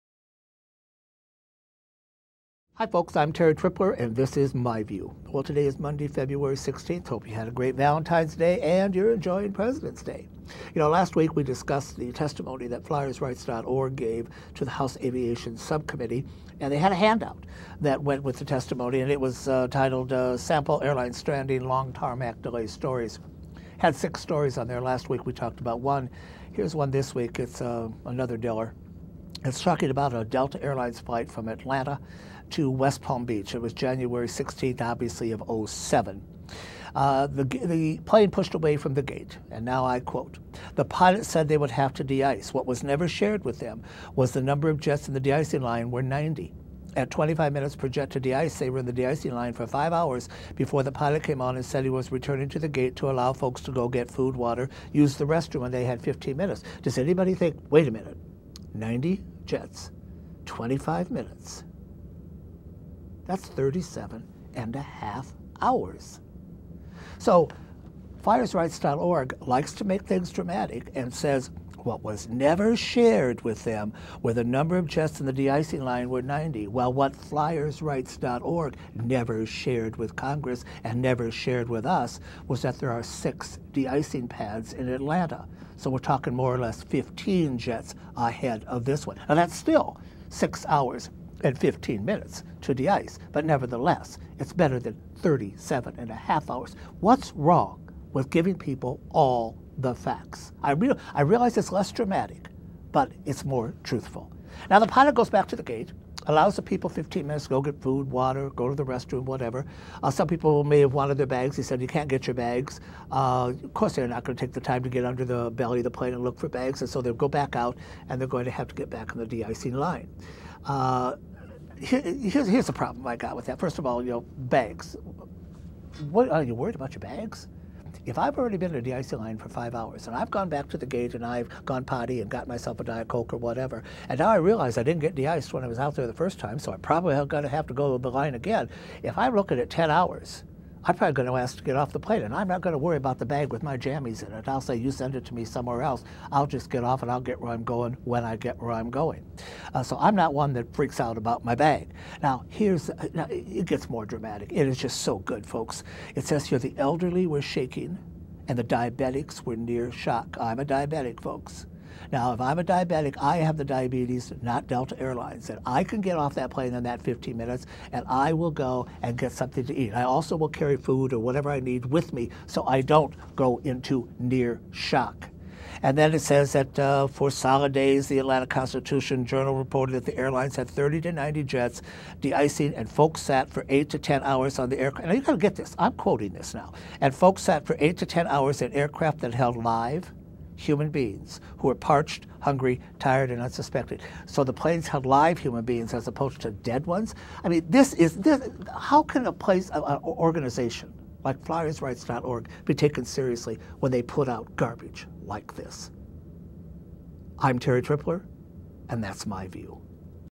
flyersrights.org and the whole story?
Again flyersrights.org was on capital hill testifying and giving out accounts of tarmac delays and obviously left out important details in the story...